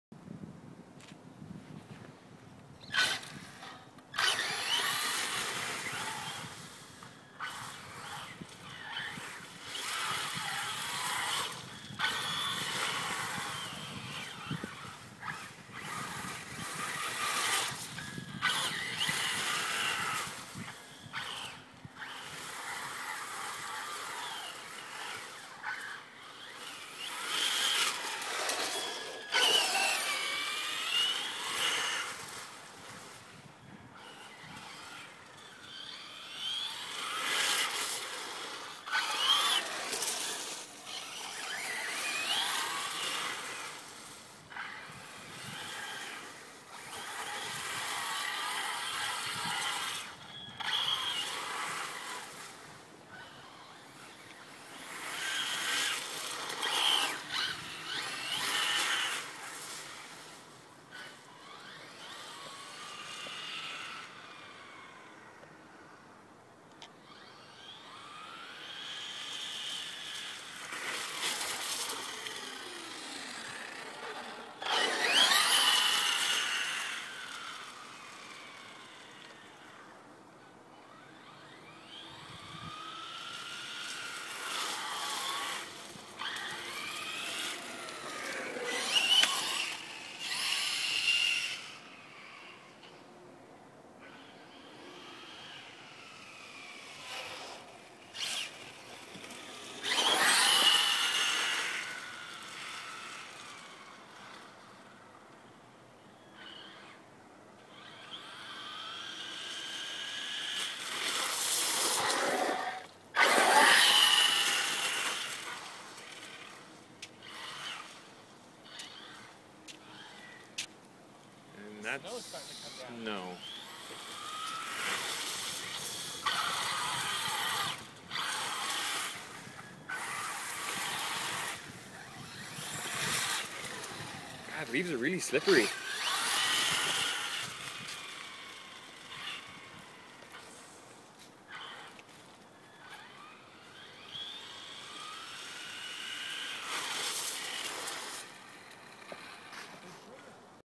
Axial Exo Terra Fall Bash
Driving through some slippery leaves.